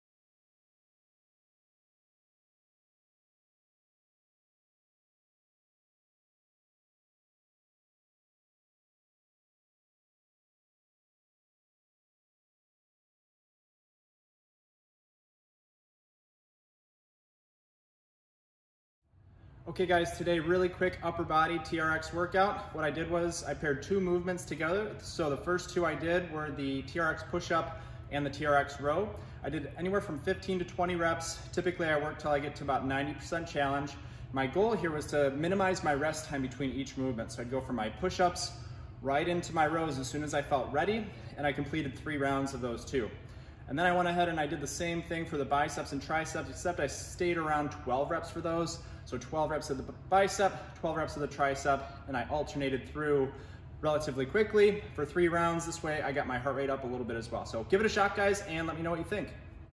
TRX Upper Body Burner
Alright guys, here's a quick upper body TRX workout. If you're pressed for time, you could use this for your entire workout, or you could add this to your routine.

Here's how it works: I grouped two movements together, and performed 15-20 reps of each of them. I repeated for 3 rounds, and minimized my rest time between each round. The idea here is to get a heart rate (cardio) component to the workout.  I recommend working until you find about a 90% challenge, so that's why there's a range of 15-20 reps. Here's how I grouped them:

Group 1:
TRX Pushups x15-20
TRX Rows x15-20
(3 rounds, minimal rest)

Group 2:
TRX Biceps x12
TRX Triceps x12

Give it a shot, and let me know what you think!